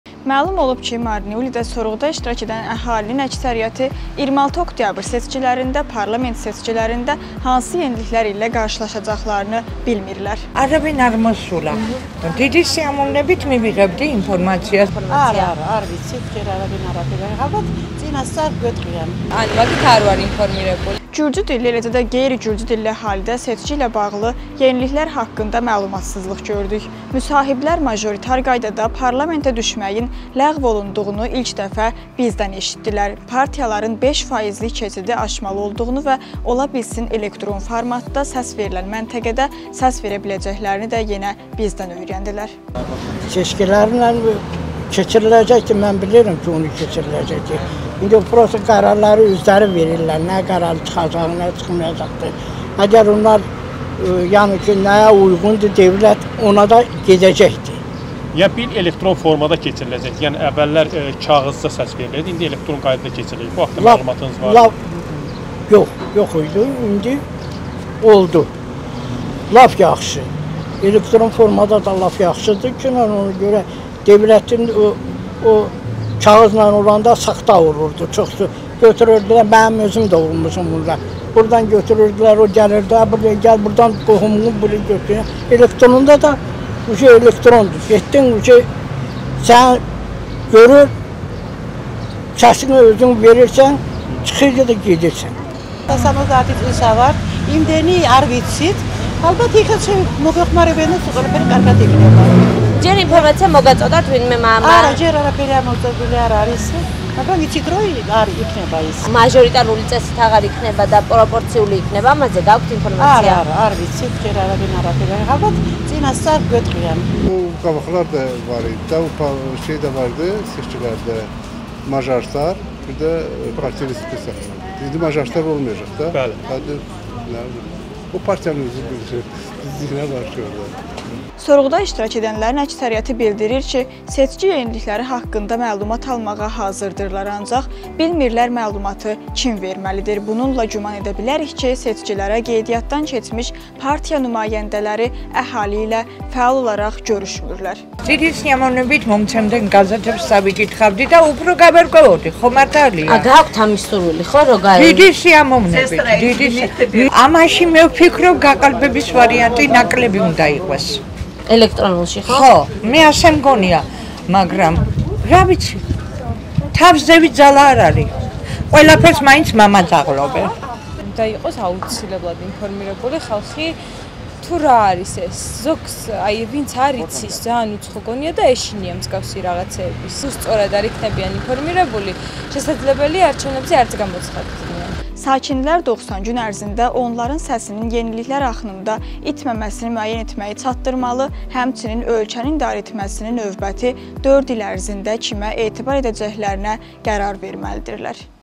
Marneulidə sekilərlə bağlı sorğu
🎬 Məlum olub ki, Marneulidə sorğuda iştirak edən əhalinin əksəriyyəti 26 oktyabr seçkilərində, Parlament seçkilərində hansı yeniliklər ilə qarşılaşacaqlarını bilmirlər.
🗳 Sorğuda iştirak edənlərin əksəriyyəti bildirir ki, seçki yenilikləri haqqında məlumat almağa hazırdırlar.